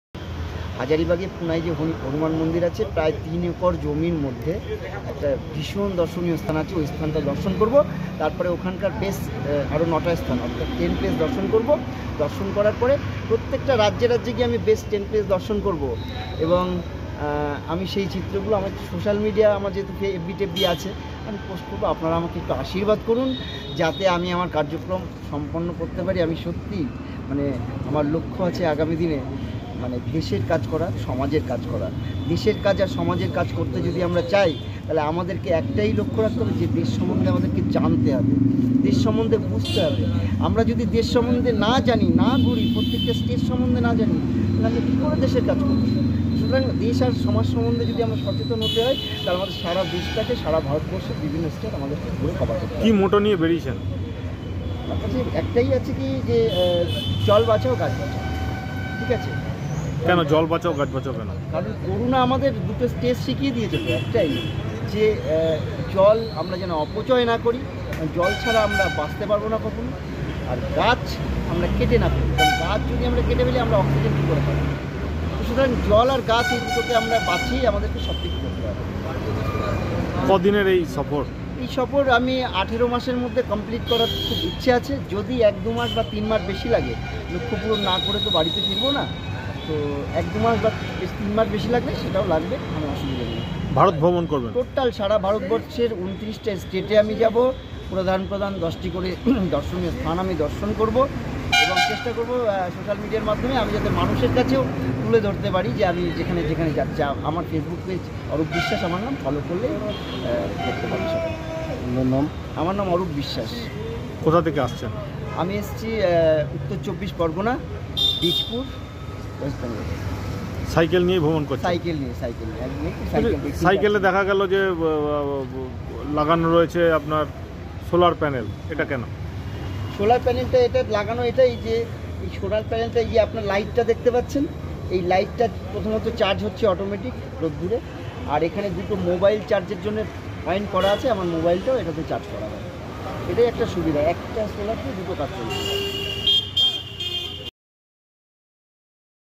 India Tour By Bicycle | জল সংরক্ষণ ও গাছ বাঁচানোর বার্তা নিয়ে সাইকেলে ভারত ভ্রমণ যুবকের
জলের অপচয় রোধ ও গাছ লাগানোর বার্তাকে সামনে রেখে নেতাজির 125তম জন্মবার্ষিকীর দিনে ভারত ভ্রমণে বেরিয়ে পড়লেন এক যুবক ৷ গত 23 জানুয়ারি নিজের দু'চাকার পরিবেশবান্ধব যান নিয়ে ভারত দর্শনে (India Tour By Bicycle) বেরিয়েছেন উত্তর 24 পরগনার কাঁচরাপাড়ার অরূপ বিশ্বাস ৷